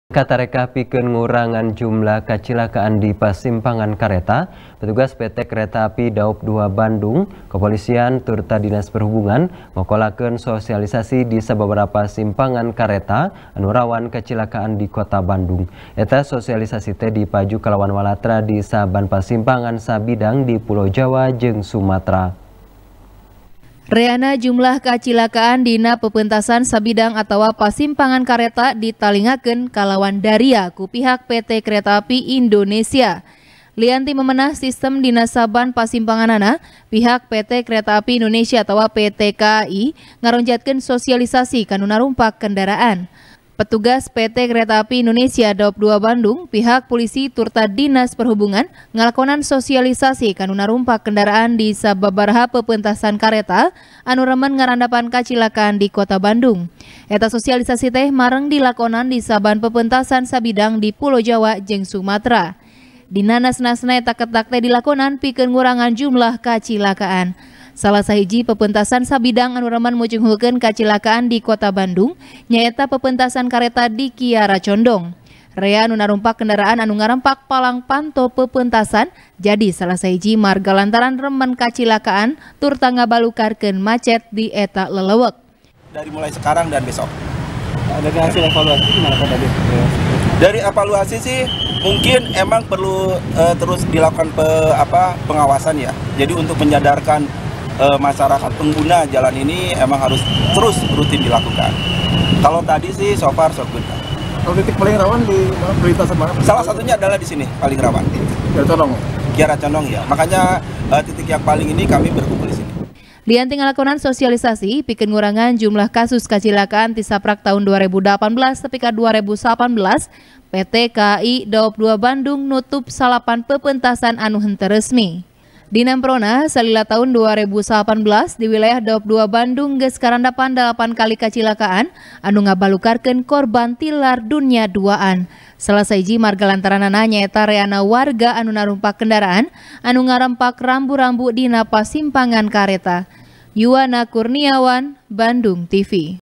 PT. Kai Sosialisasi Prakprakan Ngalalar Di Pasimpangan Kareta | TP | BANDUNGTV
TP | BANDUNGTV | IWAN M. RIDWAN | Minangka Tarekah Pikeun Ngurangan Jumlah Kacilakaan Di Pasimpangan Kareta, Petugas PT Kereta Api Daop Dua Bandung, Kepolisian Turta Dinas Perhuhungan, Ngokoalkeun Sosialisasi Di Sababaraha Pasimpangan Kareta Anu Rawan Kacilakaan Di Kota Bandung. Eta Sosialisasi Teh Dipaju Kalawan Walatra Di Saban Pasimpangan Sabidang Di Pulo Jawa Jeung Sumatera.